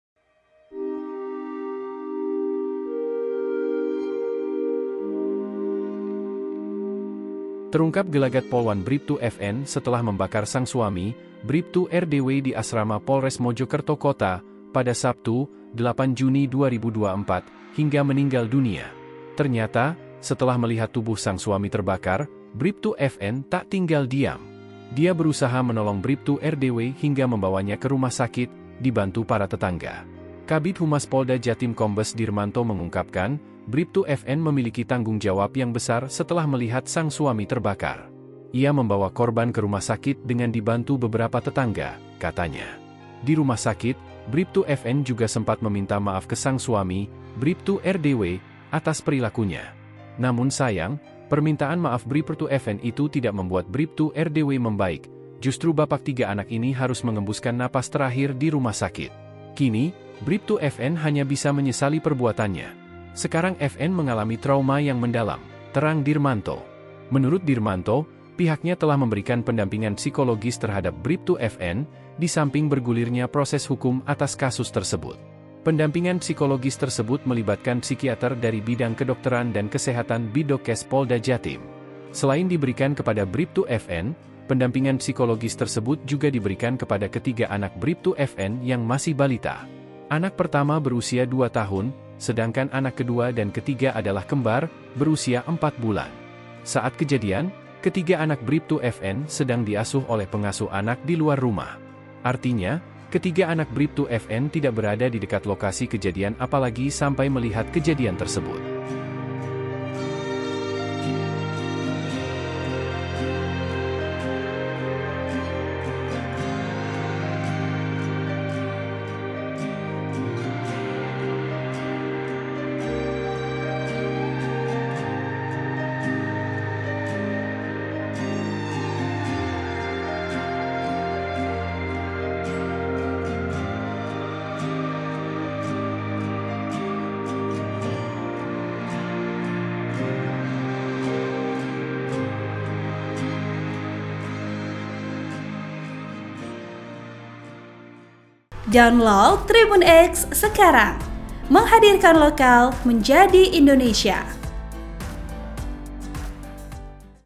Gelagat Briptu FN, Polwan Bakar Suami di Mojokerto, Antar Korban ke RS Sambil Terus Ucap 2 Kata Ini
SURYA.CO.ID - Terungkap gelagat polwan Briptu FN (28) setelah membakar sang suami, Briptu RDW (27) di Asrama Polres Mojokerto Kota, pada Sabtu (8/6/2024) hingga meninggal dunia.

Ternyata, setelah melihat tubuh sang suami terbakar, Briptu FN tak tinggal diam.

Dia berusaha menolong Briptu RDW hingga membawanya ke rumah sakit, dibantu para tetangga.

Kabid Humas Polda Jatim Kombes Dirmanto mengungkapkan, Briptu FN memiliki tanggungjawab yang besar setelah melihat sang suami terbakar.

"FN mempunyai tanggungjawab yang besar untuk menolong yang bersangkutan membawa ke rumah sakit dengan dibantu beberapa tetangga," katanya.

Di rumah sakit, Briptu FN juga sempat meminta maaf ke sang suami, Briptu RDW, atas perilakunya.

Namun sayang, permintaan maaf Briprtu FN itu tidak membuat Briptu RDW membaik, justru bapak tiga anak ini harus mengembuskan nafas terakhir di rumah sakit.

Kini, Briptu FN hanya bisa menyesali perbuaannya.

"Sekarang FN mengalami trauma yang mendalam," terang

Menurut Dirmanto, pihaknya telah memberikan pendampingan psikologis terhadap Briptu FN, disamping bergulirnya proses hukum atas kasus tersebut.

Pendampingan psikologis tersebut melibatkan Psikiater dari Bidang Kedokteran dan Kesehatan Biddokkes Polda Jatim.

Selain diberikan kepada Briptu FN, pendampingan psikologis tersebut juga diberikan kepada ketiga anak Briptu FN yang masih balita.

Anak pertama berusia dua tahun, sedangkan anak kedua dan ketiga adalah kembar, berusia empat bulan.

"Dia masih trauma mendalam. Sekarang sedang ditangani dan sedang difasilitasi untuk trauma healing oleh Polda Jatim, kemudian juga kita melibatkan psikiater untuk menangani kasus ini," ujarnya saat dihubungi TribunJatim.com, Senin (10/6/2024).

Saat kejadian penganiayaan terjadi, ketiga anak Briptu FN sedang diasuh oleh pengasuh anak di luar rumah.

Artinya, ketiga anak Briptu FN tidak berada di dekat lokasi kejadian apalagi sampai melihat kejadian tersebut.

Artikel ini telah tayang di Surya.co.id dengan judul Gelagat Polwan Briptu FN Usai Bakar Suami di Mojokerto, Antar ke RS Sambil Terus Ucap 2 Kata Ini,
Penulis: Luhur Pambudi | Editor: Musahadah